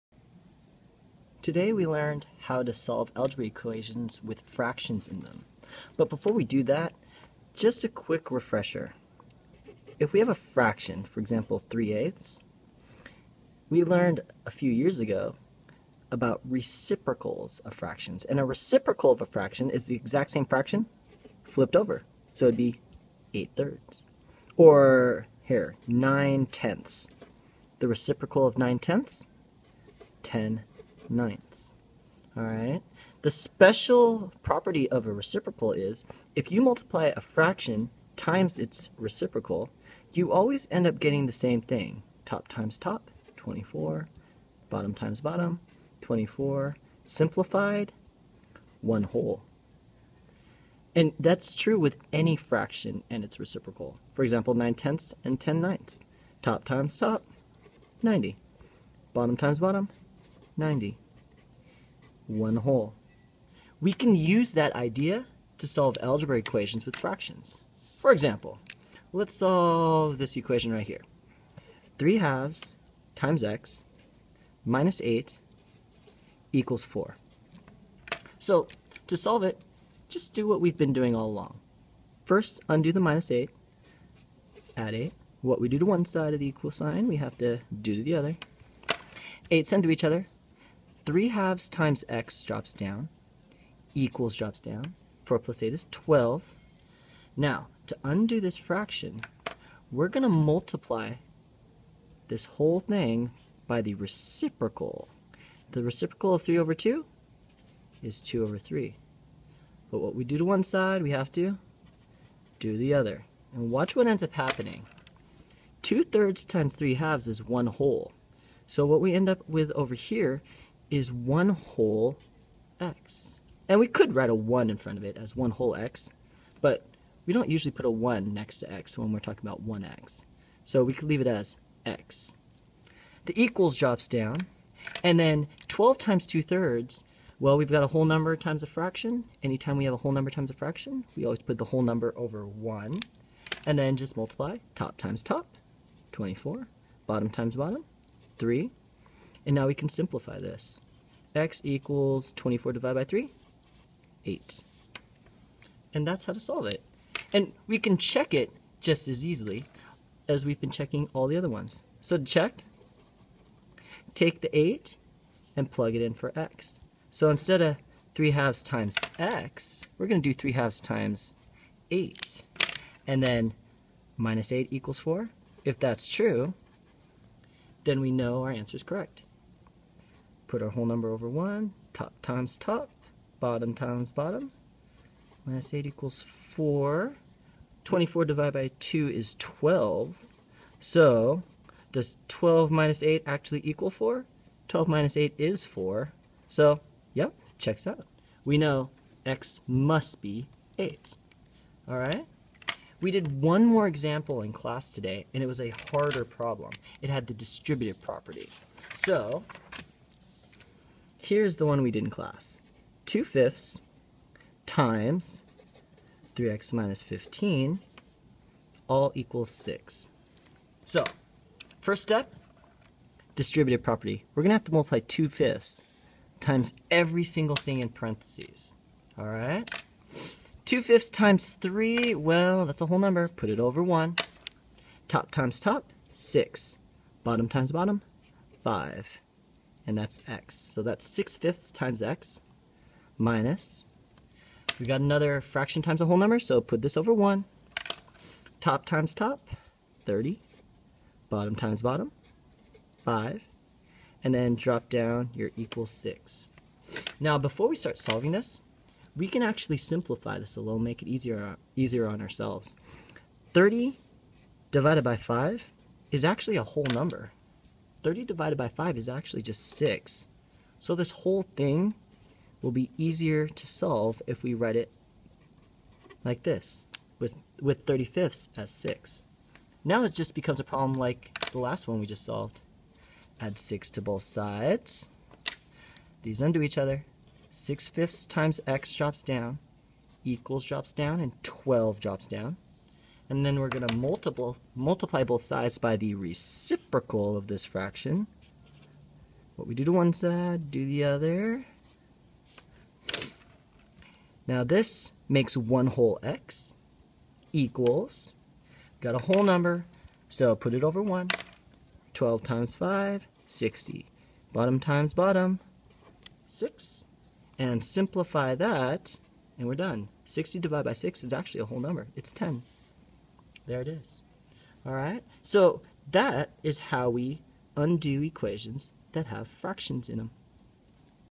Undoing Fractions in Algebra Equations
Algebra can be hard!  In this video, I show you how to solve algebra equations with fractions in them!